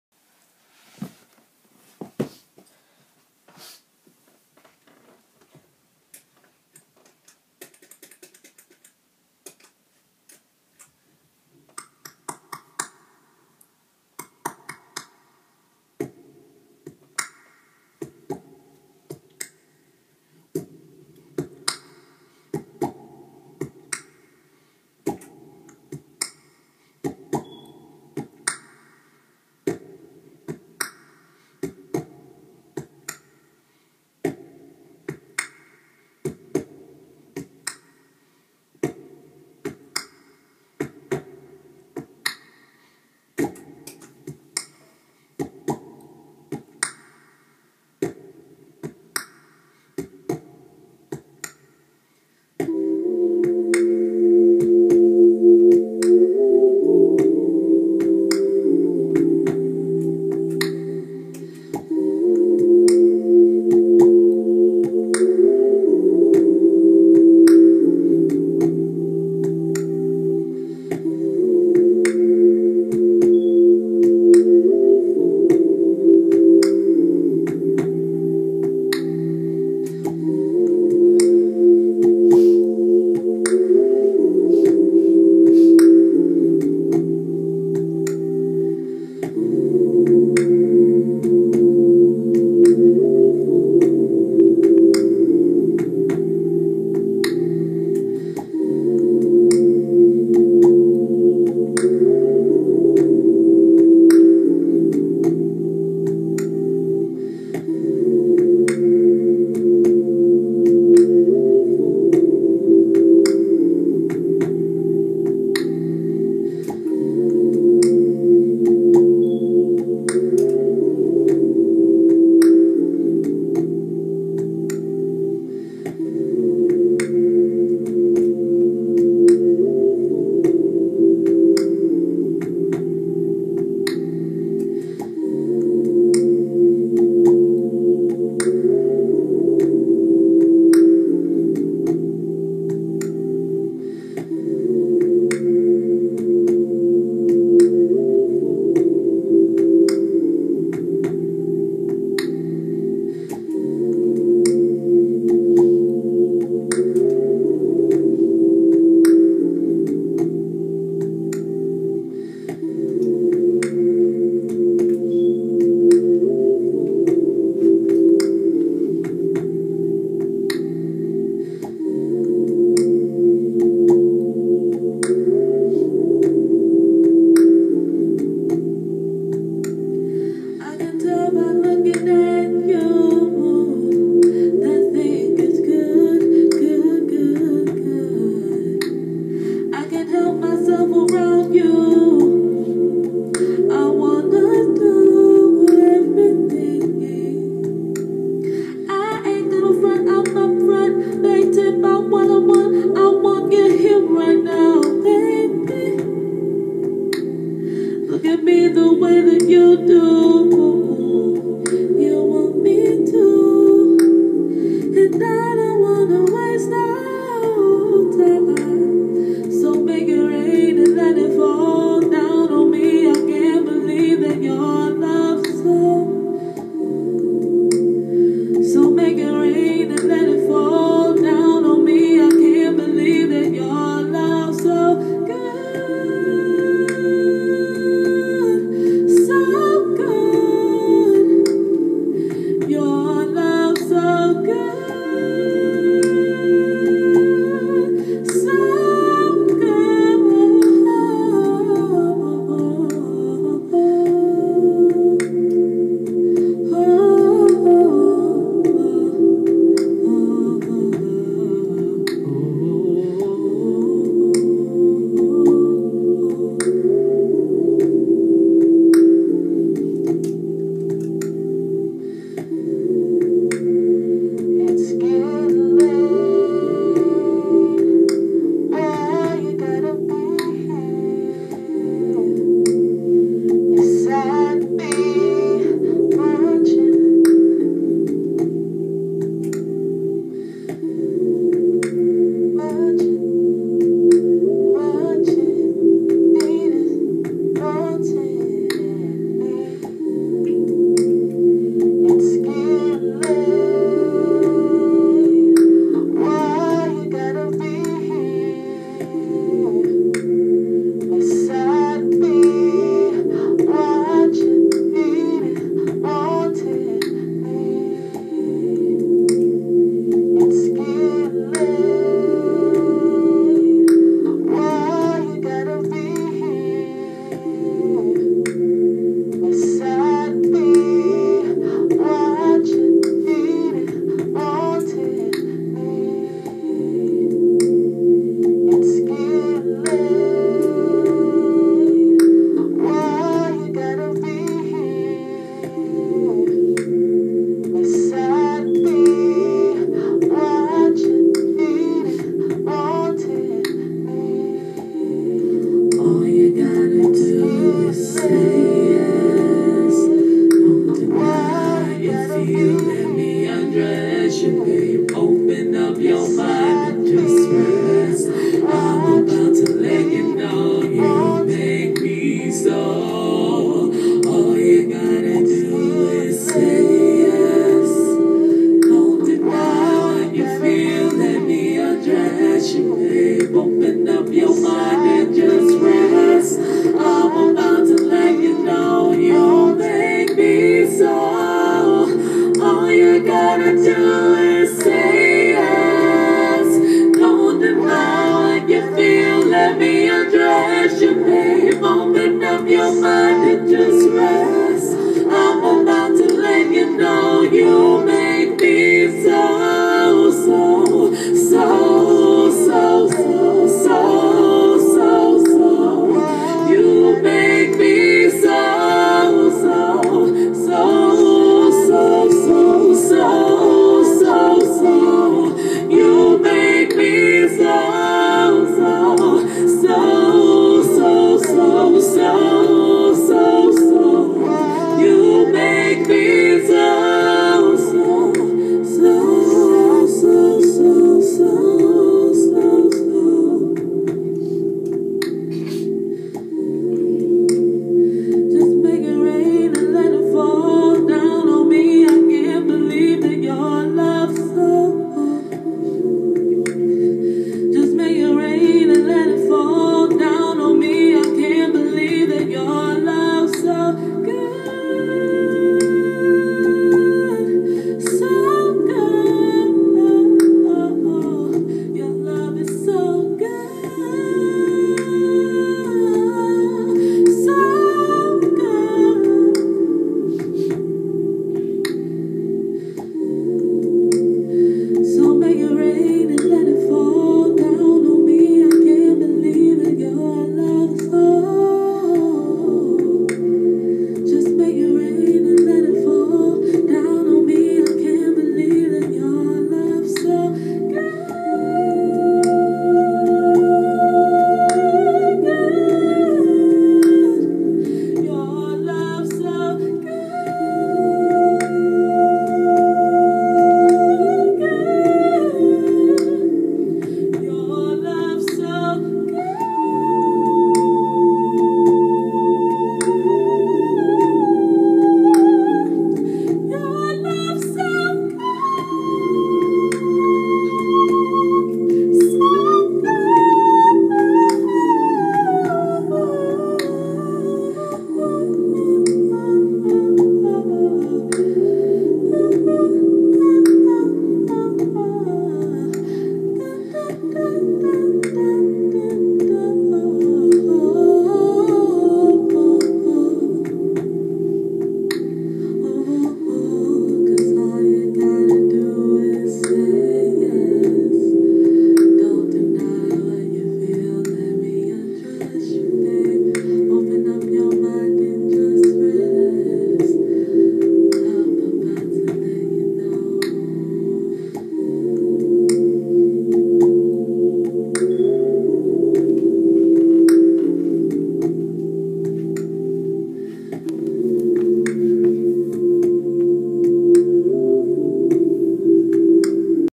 Marsha Ambrosius Medley (Live Looping Cover)
I usually rehearse and practice a build before I record but this time I just decided to hit the record button and let my own imagination lead. Medley of my fav songs sung by Marsha Ambrosius... Enjoy! Twitter: @jacqconmusic instagram: @jacqconmusic88 DopeAzzMusic 2014